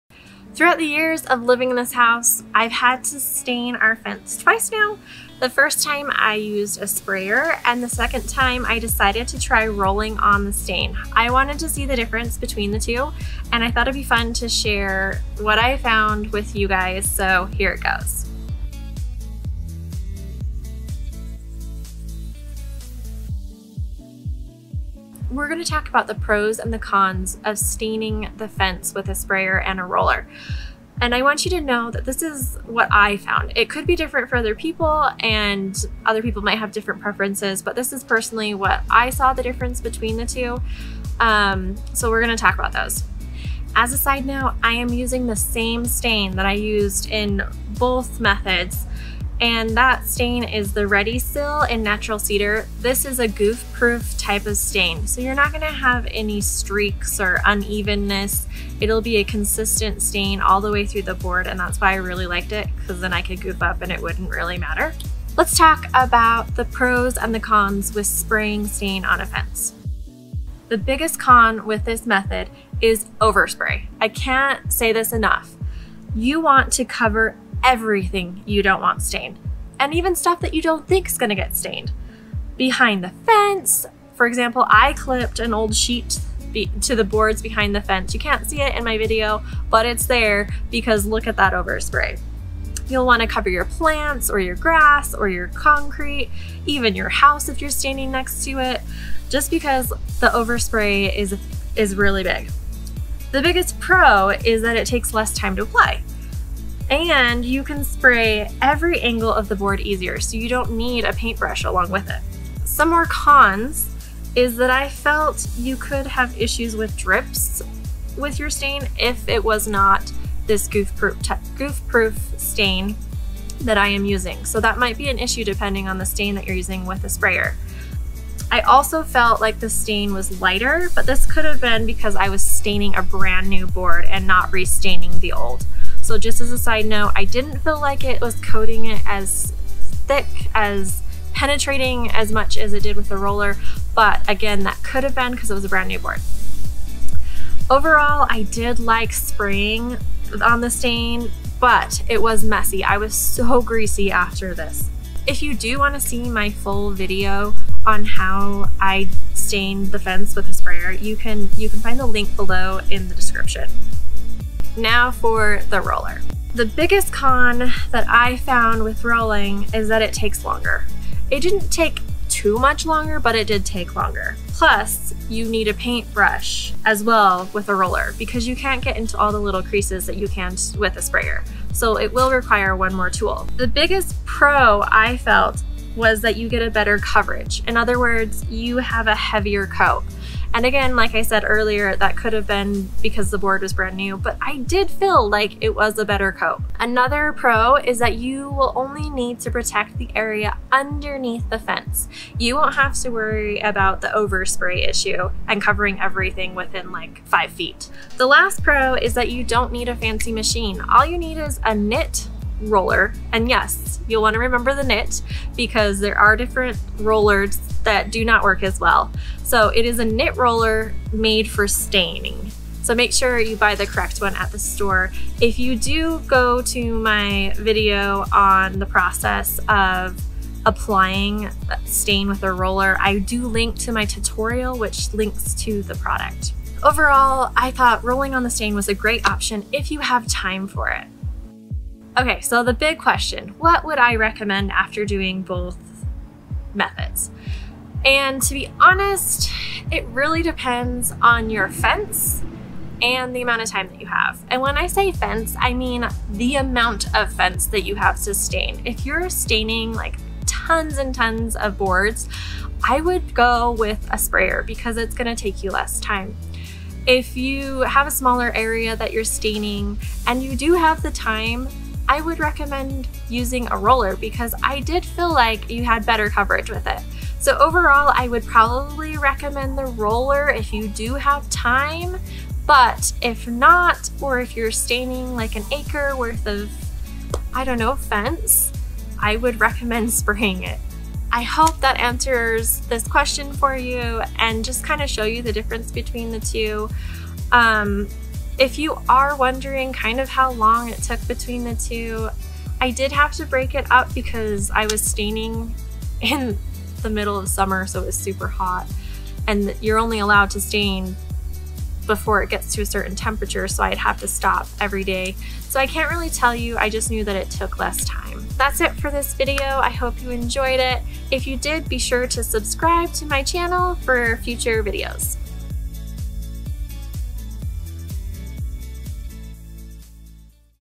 Difference in Staining a Fence by Spraying and Rolling
Are you wondering what is the best way to stain a wooden fence? Spray or roll?

In this video, I share what I found after trying both methods. Plus I share the pro's and con's I personally found between the two.

Watch the video to see what I would recommend.

- Ready Seal in Natural Cedar
- Critter paint sprayer
- Safety glasses